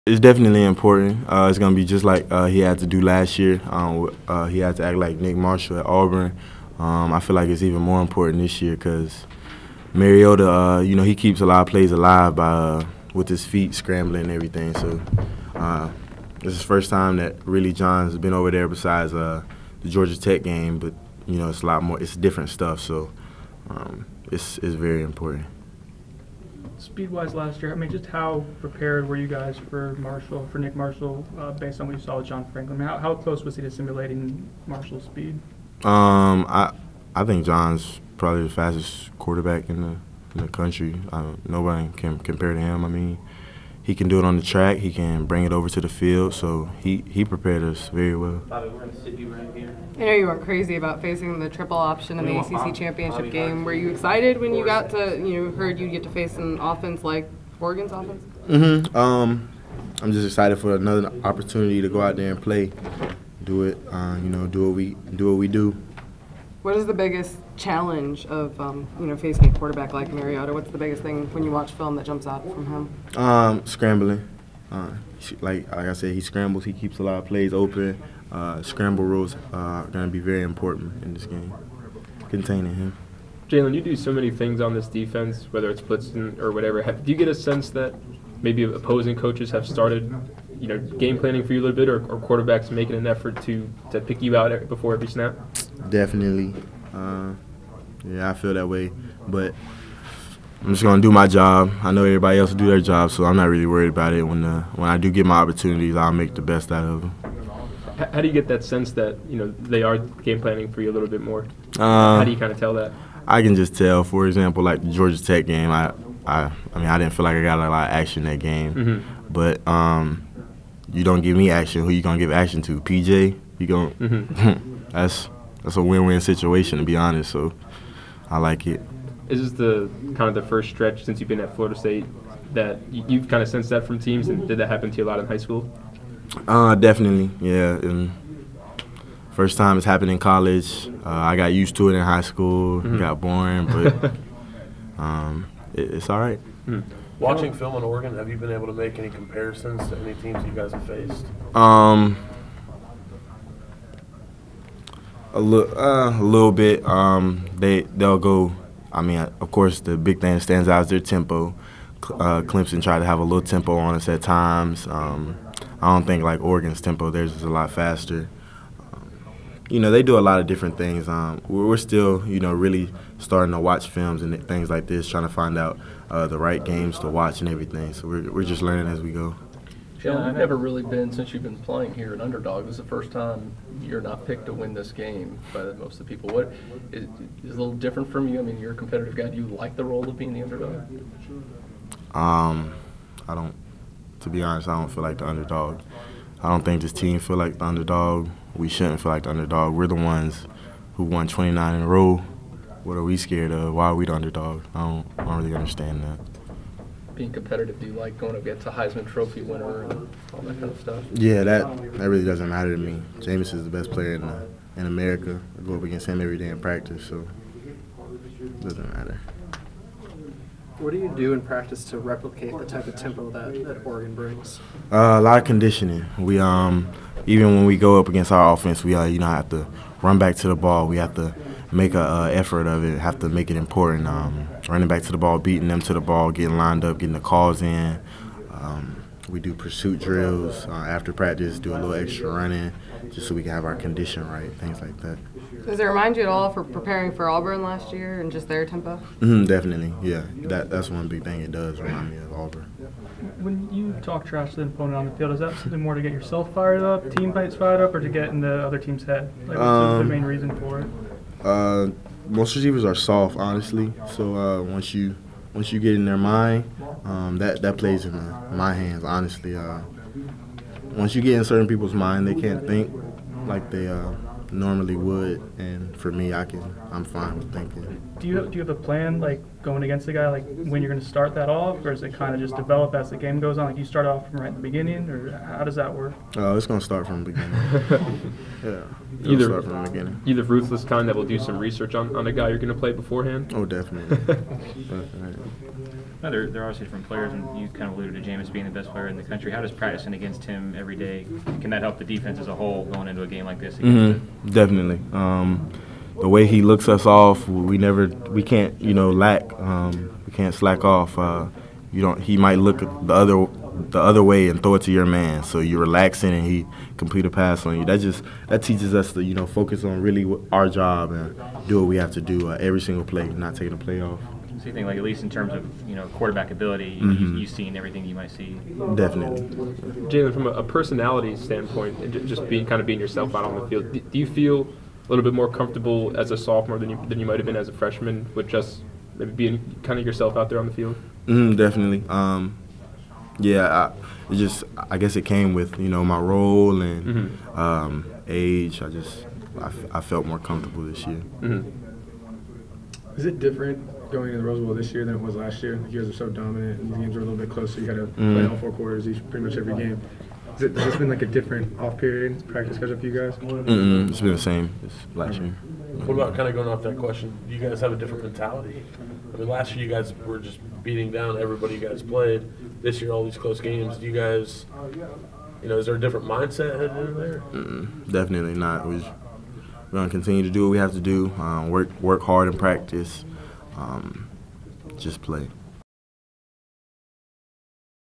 Jalen Ramsey Interview: December 22
Courtesy Florida State safety Jalen Ramsey meets with the media.